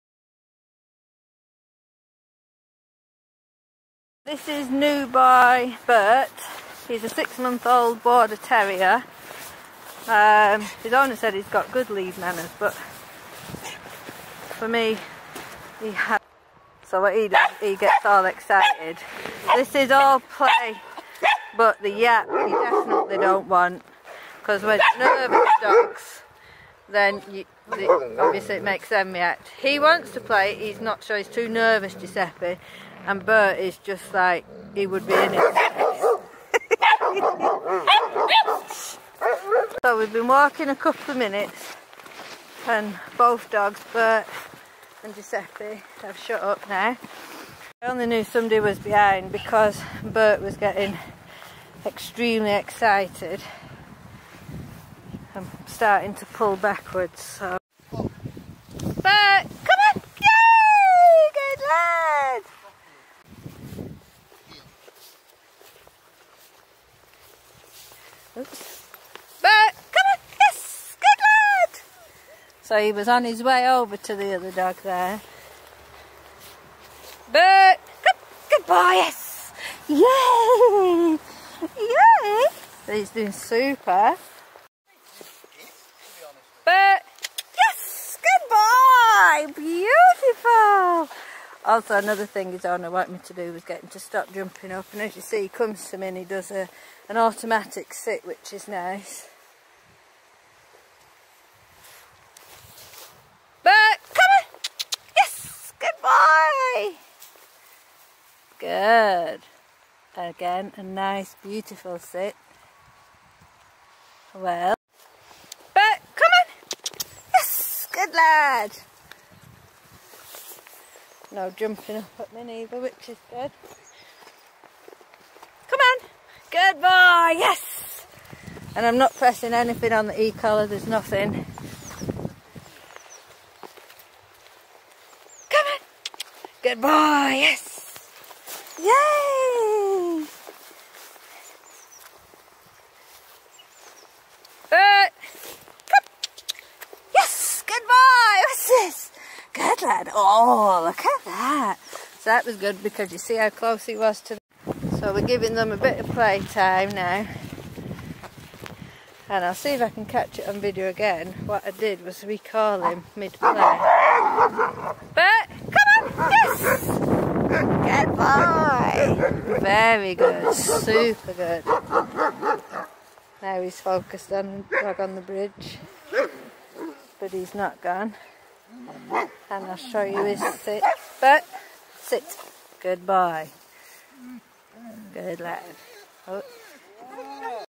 6 month old Border Terrier Burt, day 1 of 5 day boot camp
Burts in for basic obedience, today I work on sit and recall. he does great ❤️